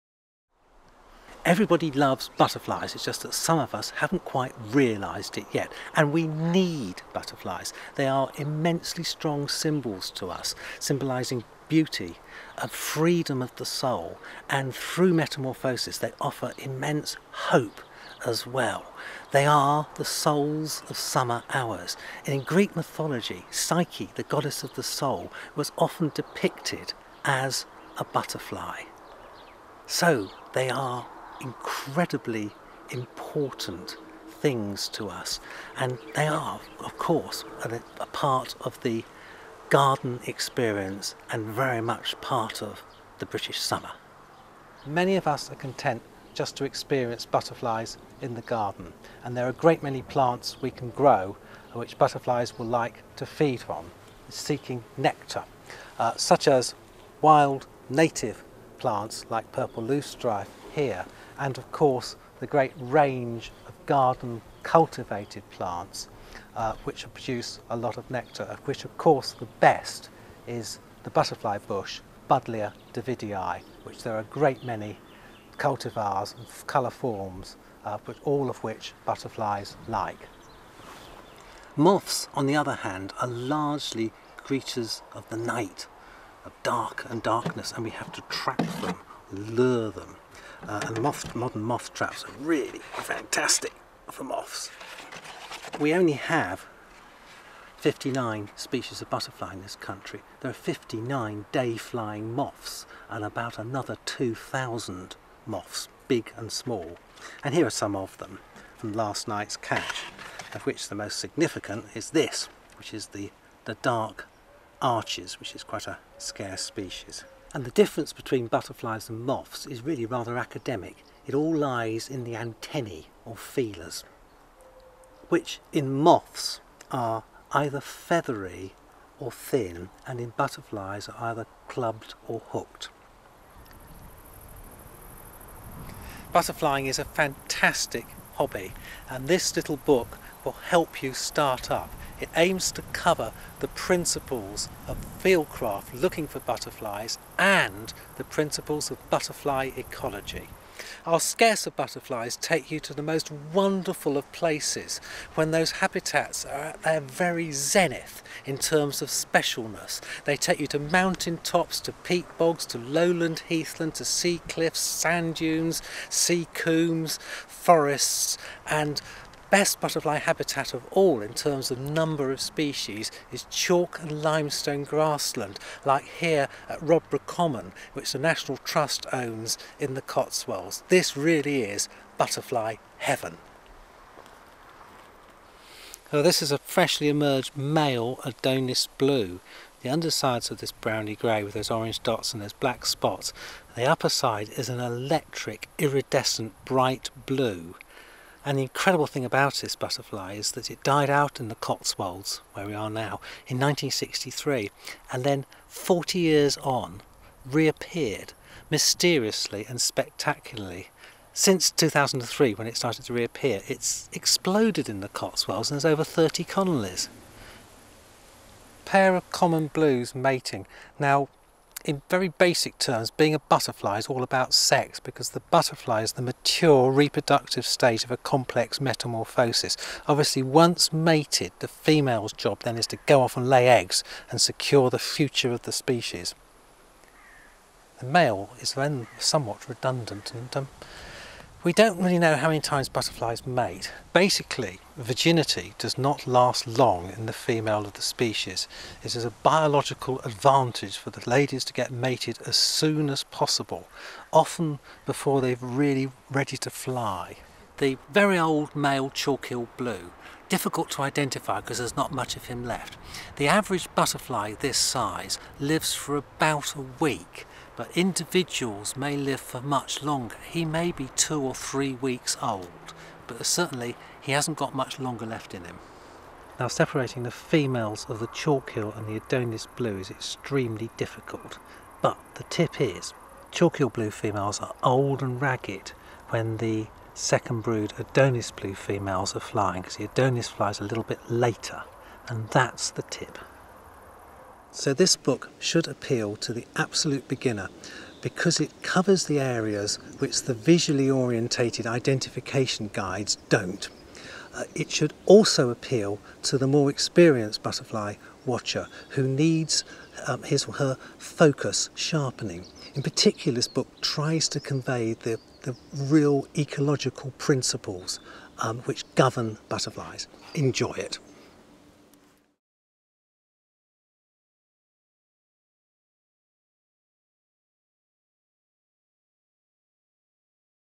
Butterflies: Spotting and identifying Britain's butterflies
Spotting and identifying Britain's butterflies.

Presented by Matthew Oates, National Trust Butterfly Specialist.

by Matthew Oates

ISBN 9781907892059